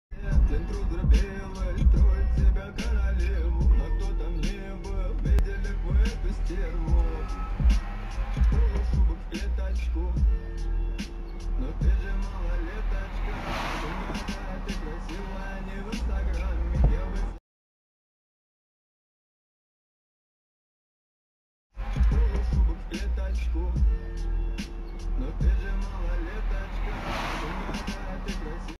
Видео с Пирятина, спортивный Рено Ам завершает маневр. Видео Николай. Так уже можно регики?
Видео с Пирятина, спортивный Рено Ам завершает маневр. Видео Николай. Так уже можно регики или ещё нет?

Видео и фото присылайте на Viber Telegram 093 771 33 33,